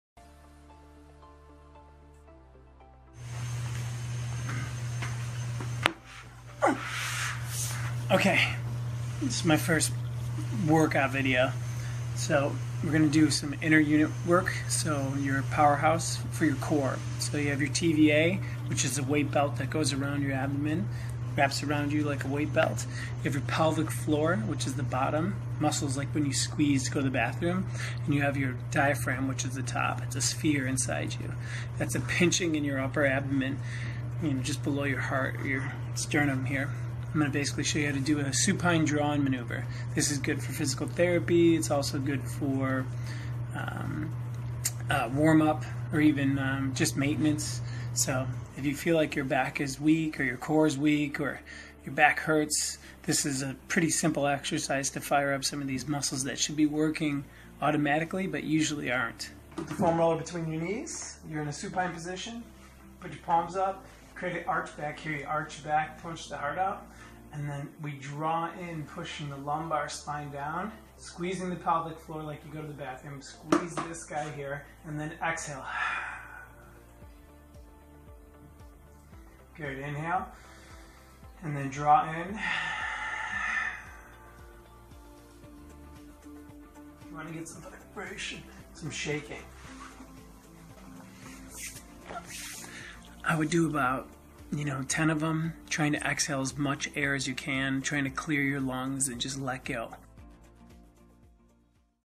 Supine Draw-in Maneuver
Fire up your inner core for pain relief, rehabilitation and warming up before you train.